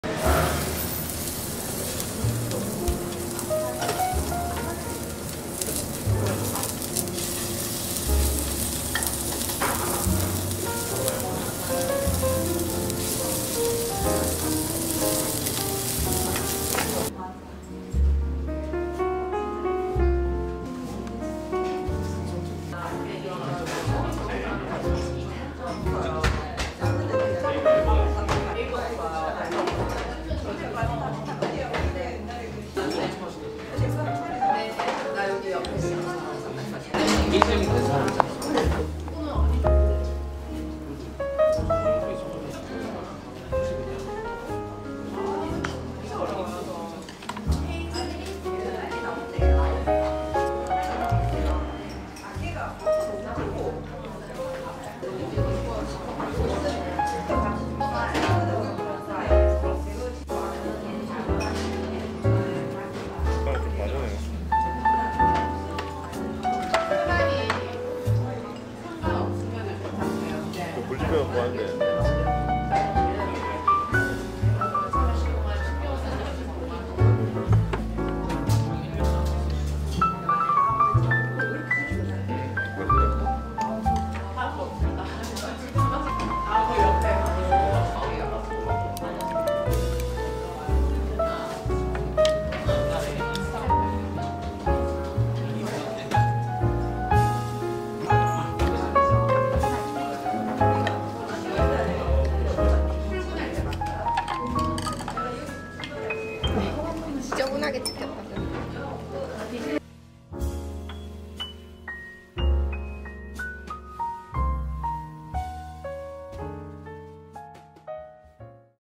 울산 덮밥 맛집 / 한그륵, 울산 유일한 간장 숯불 닭고기 덮밥 "토리동"
🚩장소 정보

한그륵 전화번호 & 주소:
울산 남구 돋질로220번길 15
울산 남구 달동 1347-1

👦Guest: 이자까야

울산in 블로그에서 더 많은 정보를 얻을 수 있어요💕

Camera Equipment Used :
-EF-M 22mm Lens
-Rode VideoMicro